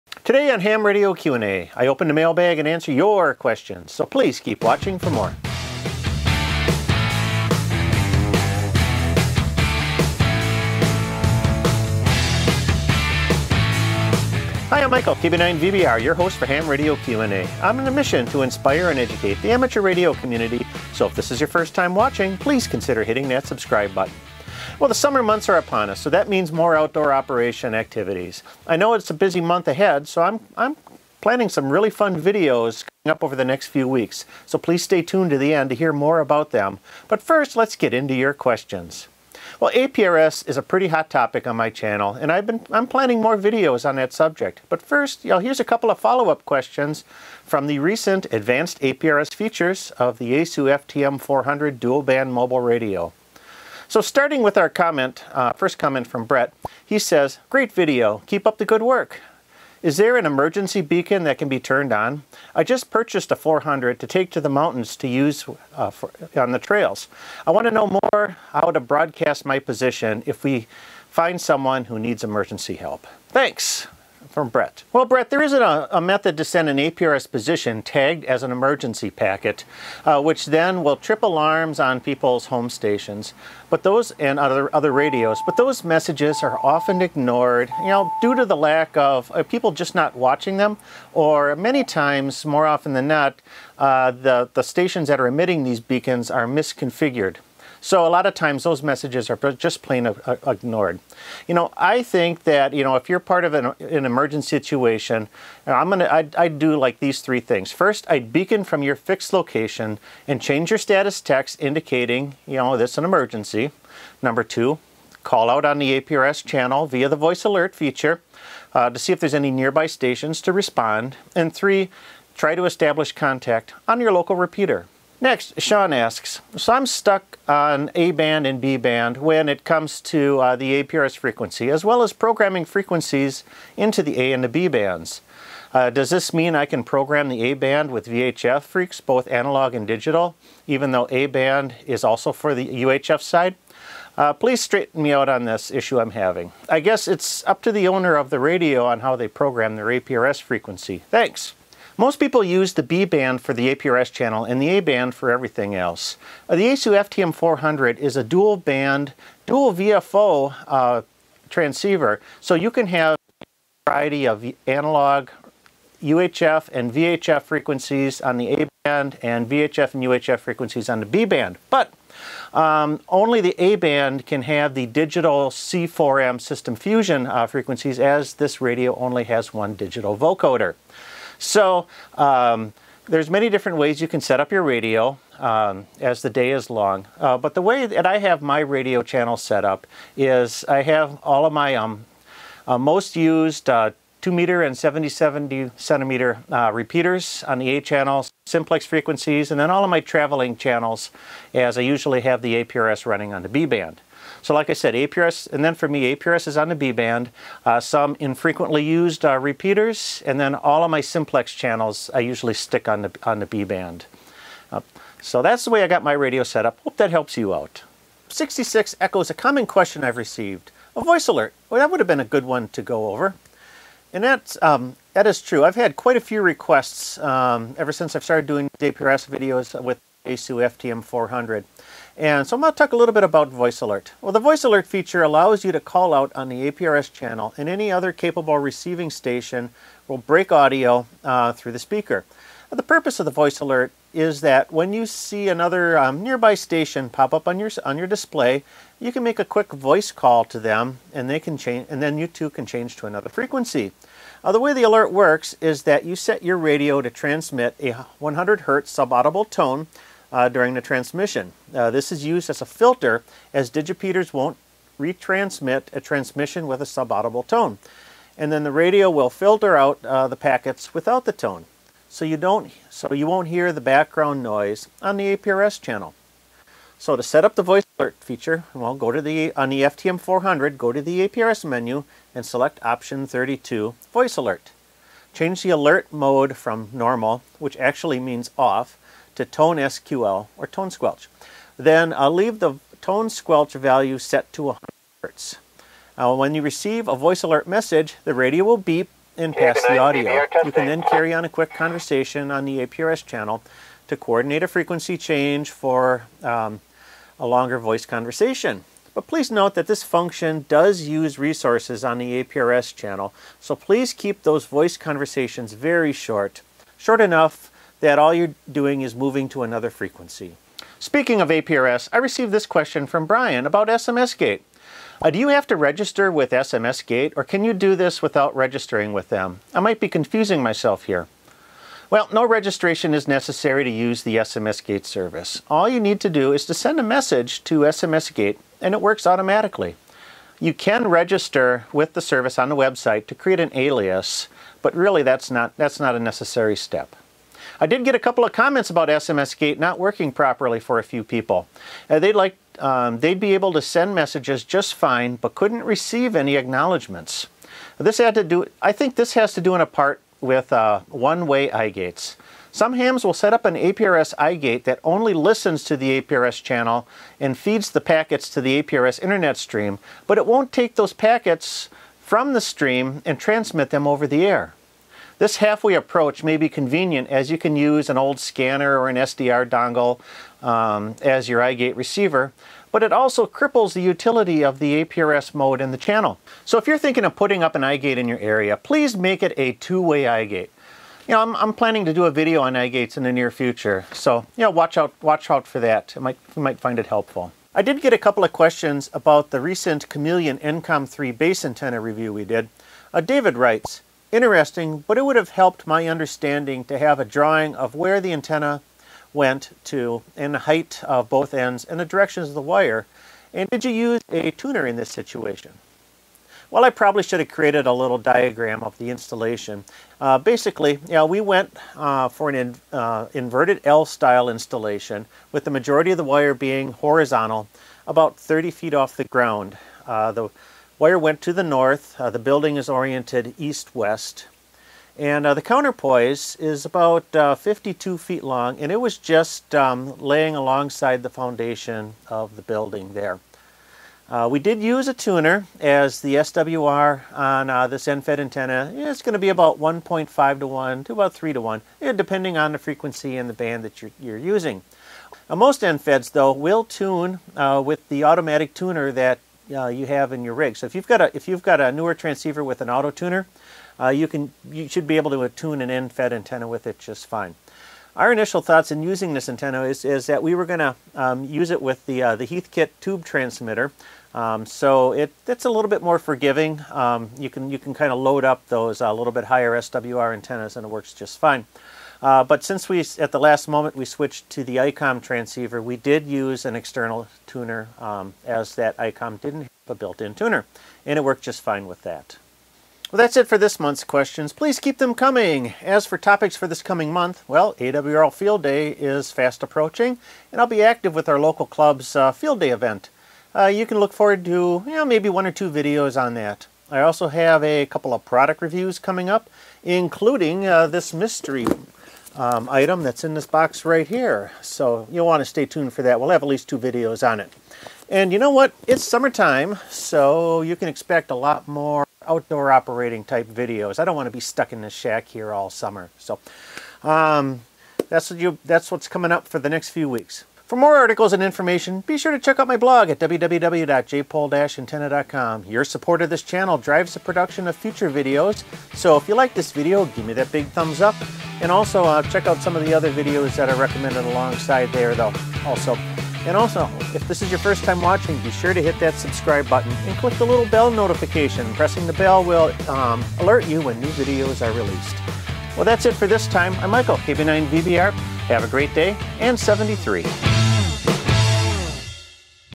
Your Questions Answered, June 2019 - Ham Radio Q&A
The summer months are upon us, so that means more outdoor operations activities. I know I have a busy month ahead so I’m planning some fun videos over the next few weeks, stay tune to the end to hear more about them. but first, let’s get into your questions.

APRS Emergency Beacon 00:58
Channel Organization 02:18
APRS Voice Alert 03:40
SMSGTE Followup 05:27
Chameleon EMCOMM III Followup 06:42